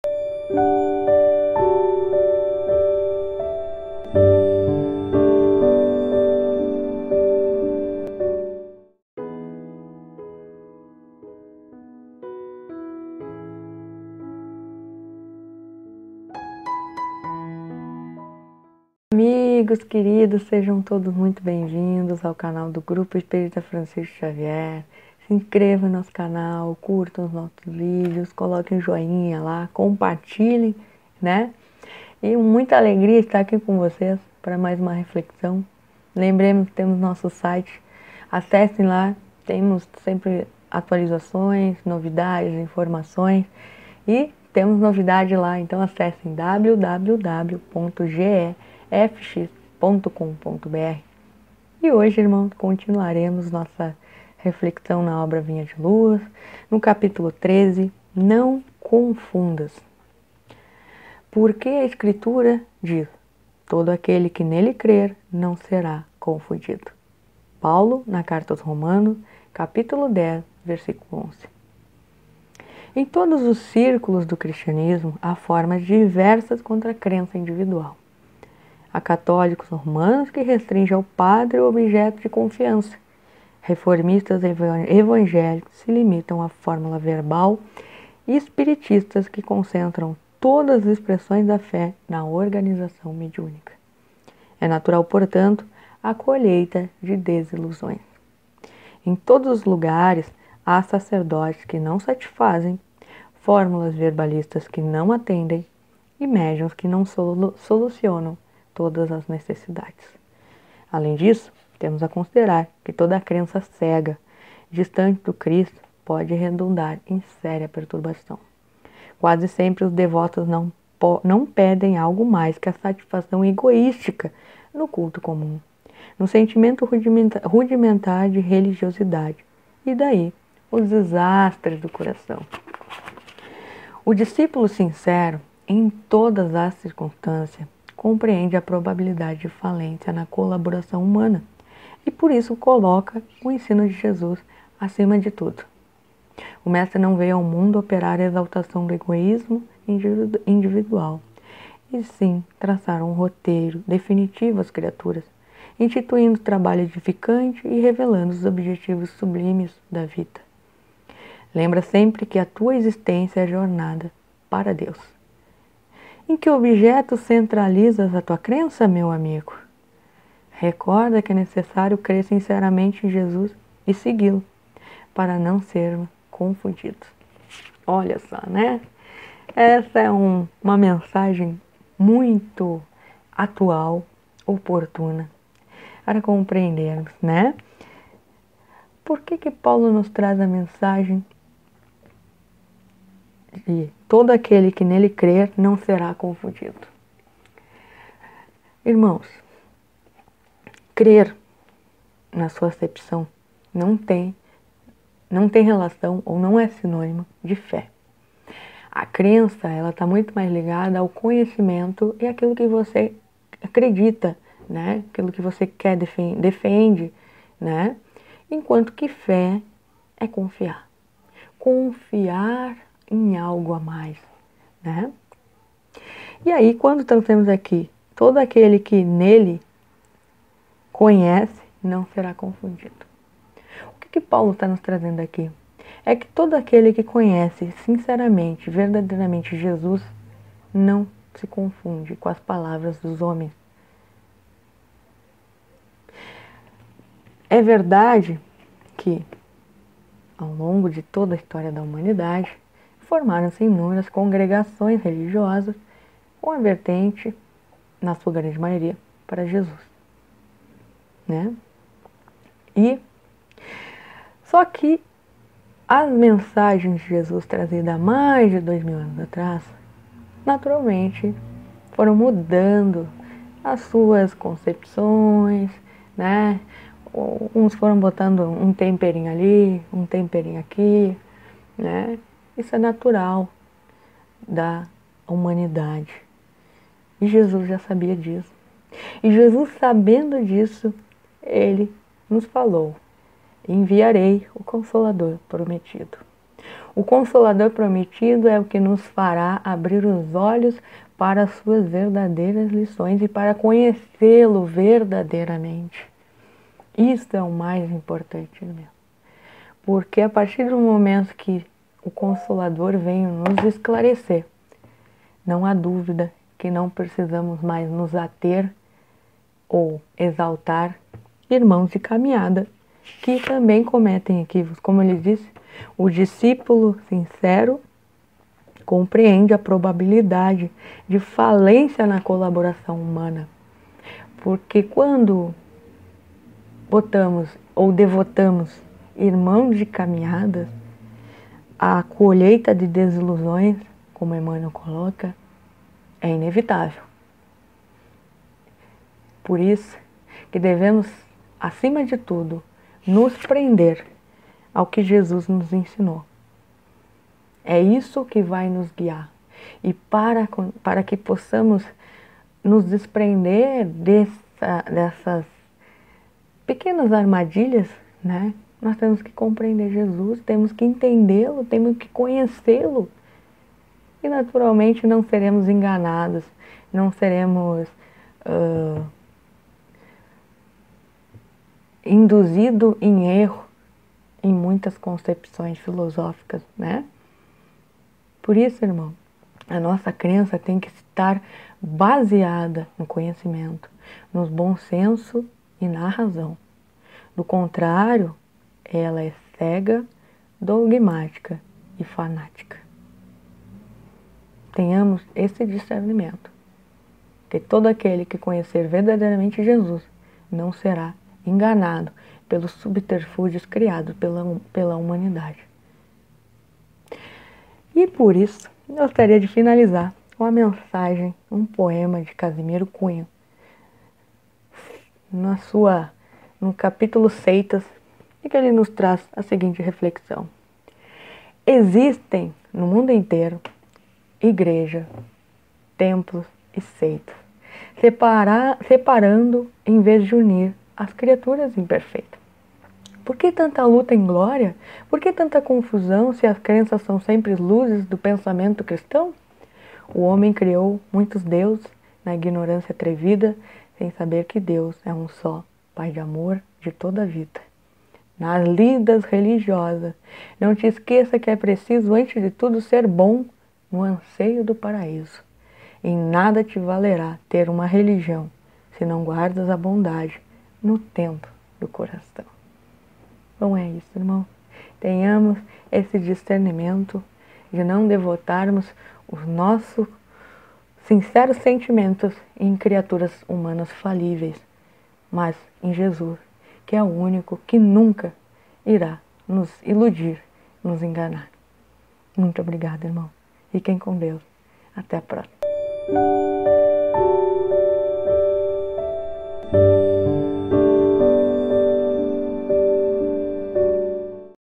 Vinha de luz - Não confundas - Lidiane Escobar
Reflexões diárias, por Lidiane Escobar, Trabalhadora do Grupo Espírita Francisco Xavier, texto retirado do livro Vinha de Luz, psicografado por Francisco Cândido Xavier, pelo Espírito Emanuel, Cap. 13 Não confundas.